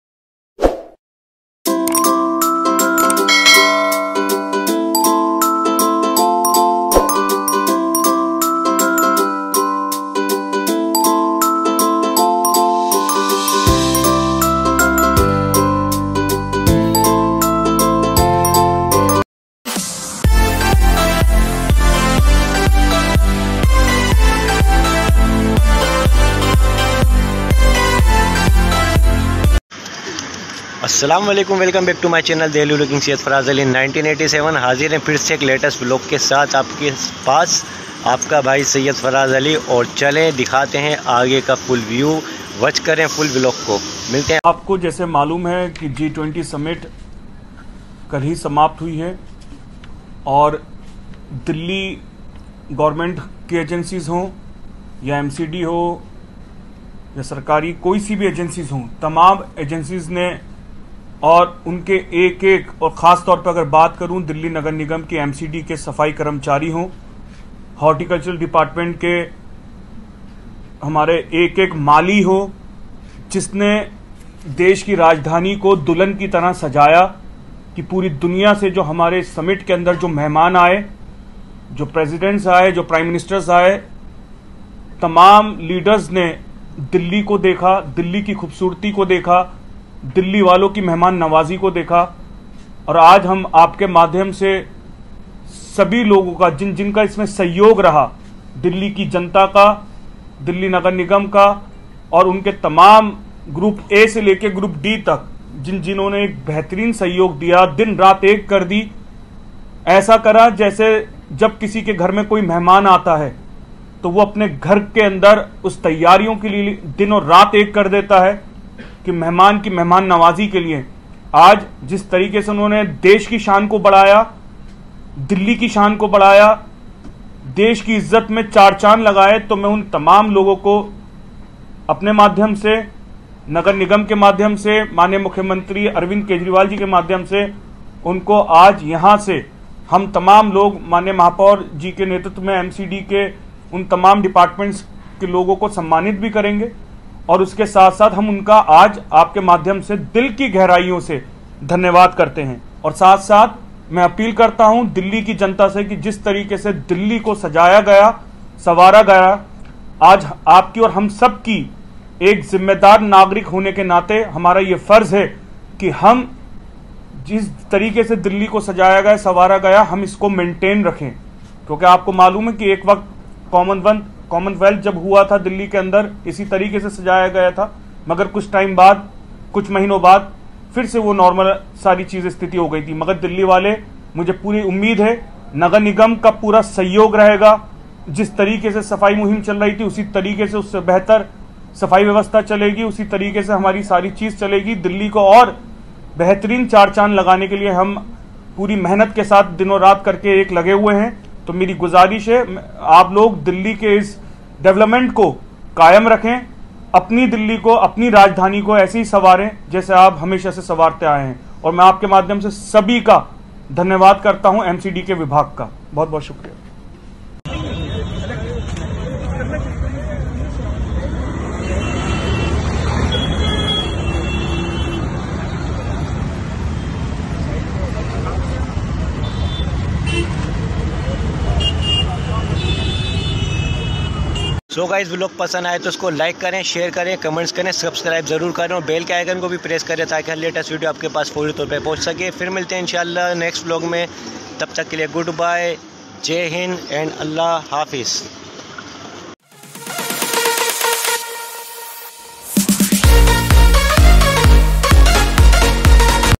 jammasjid g20 k bad kamo lekar charcha karte hua aale mohummad Iqbal❤️
Created by InShot:September 12,live update mcd k duwara kiyegaye kamo ko hosla afzai karte hua aale mohummad Iqbal❤️